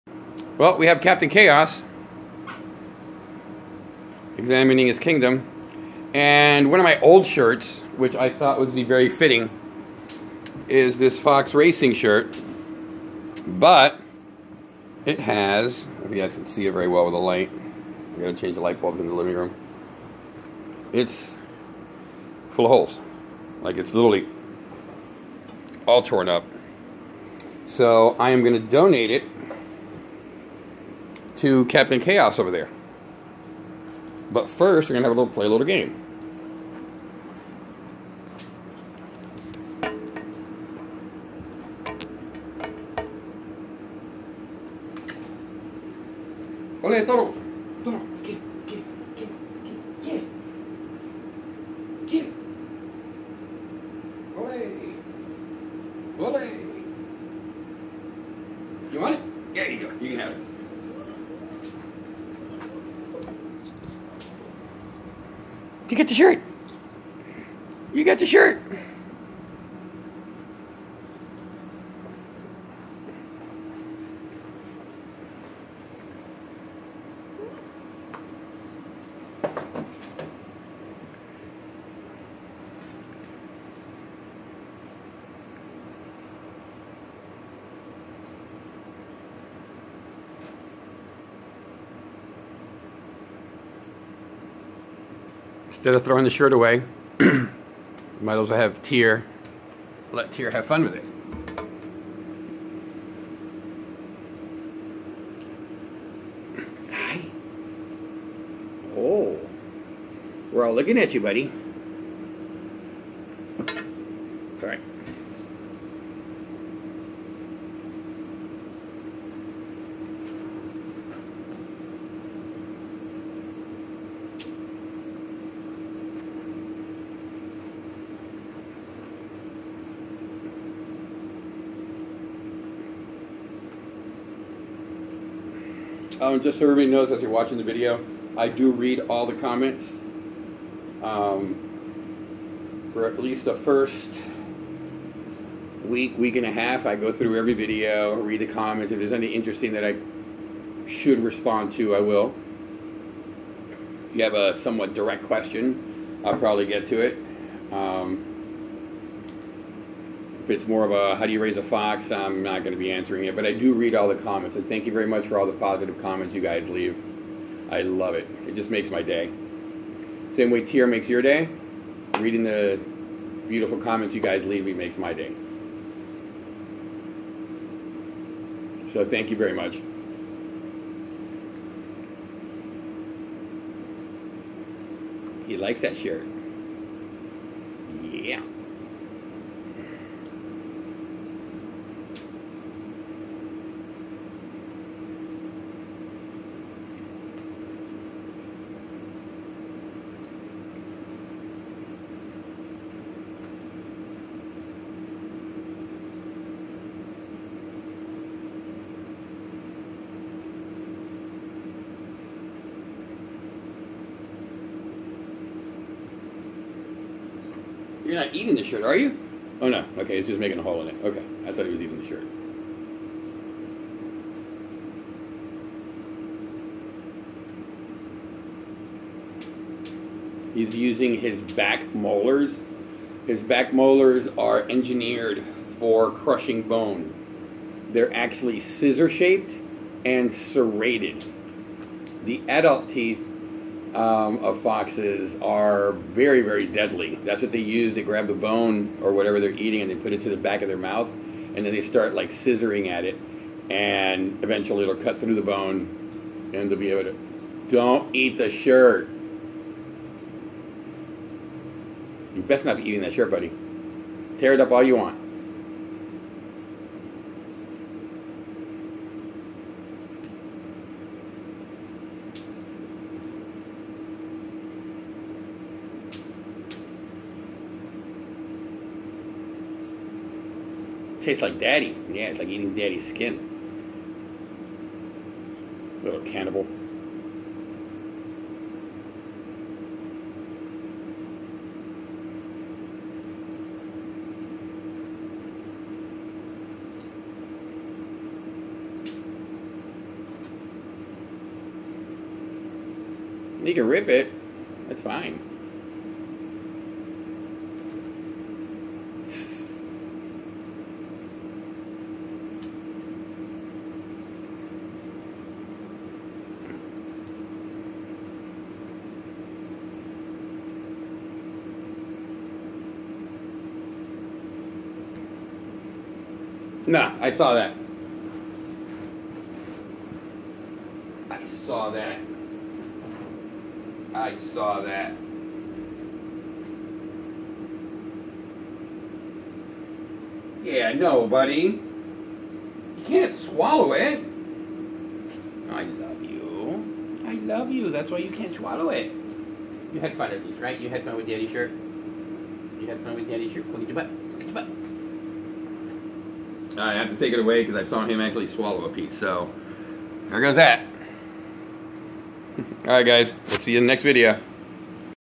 Gave TYR my fox shirt
The fox racing shirt i have wad full of holes so i decided to give it to TYR before i threw it away.  I thought he would have fun with it for a while but he decided to start eating it so I had to take it away